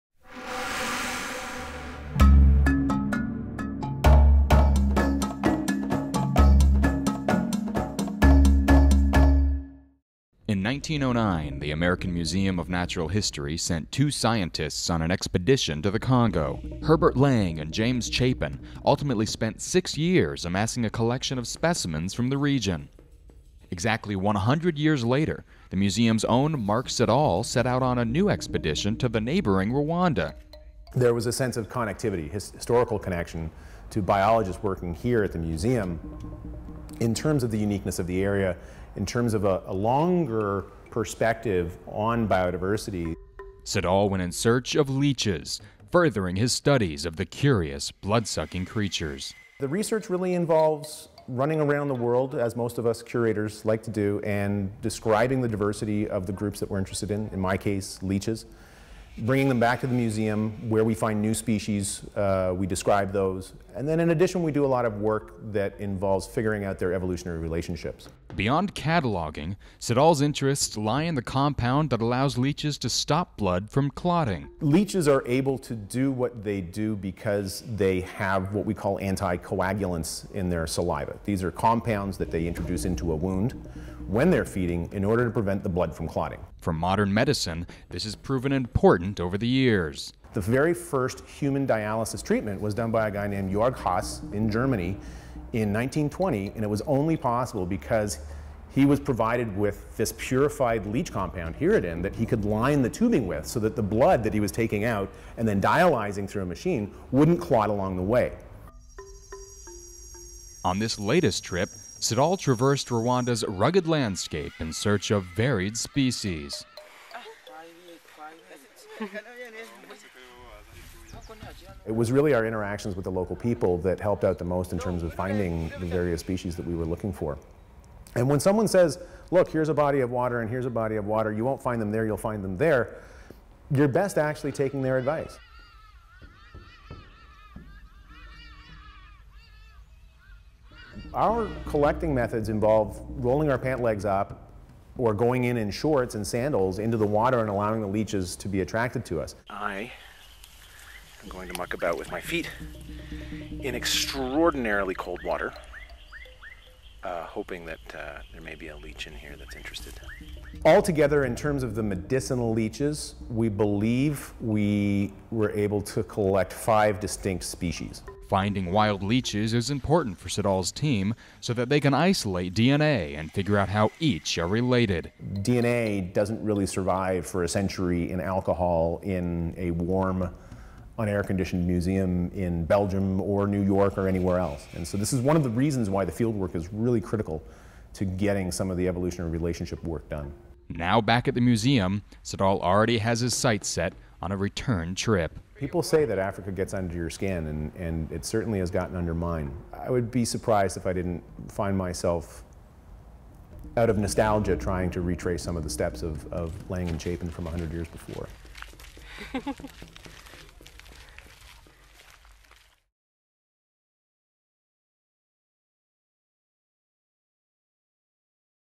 Finding Leeches in Rwanda
In 1909 the American Museum of Natural History's Herbert Lang and James Chapin embarked on a scientific expedition to the northeastern Belgian Congo. Their trip would ultimately last five and a half years yielding significant zoological and anthropological findings for the museum.

Exactly 100 years later another museum scientist embarked on an expedition to the neighboring Rwanda. As a curator of invertebrate zoology he travelled to Rwanda in search of leeches—an animal that Lang and Chapin brought back amongst their original findings.